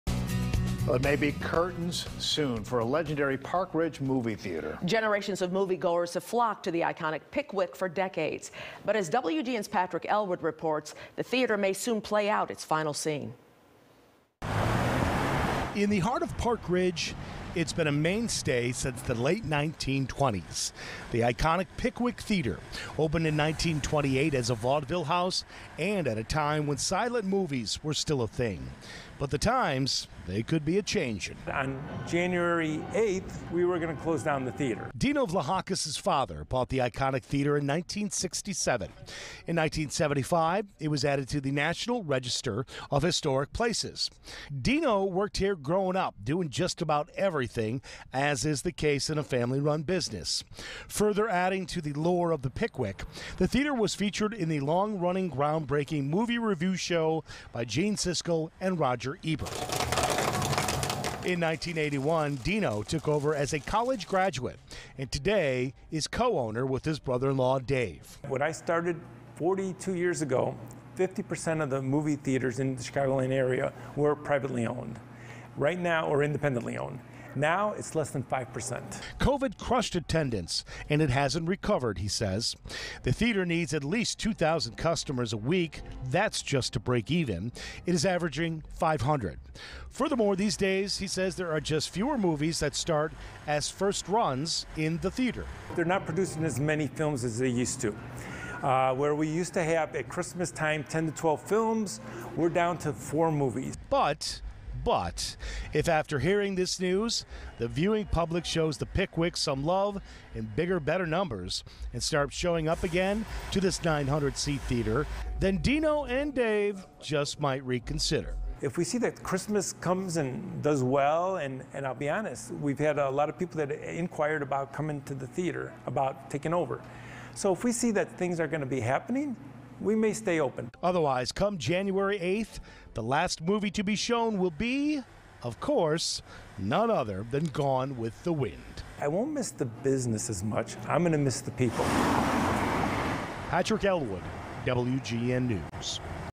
Park Ridge's iconic Pickwick Theatre closing its doors
Fewer movies are making theatrical runs, which means less ticket sales, the business owner said.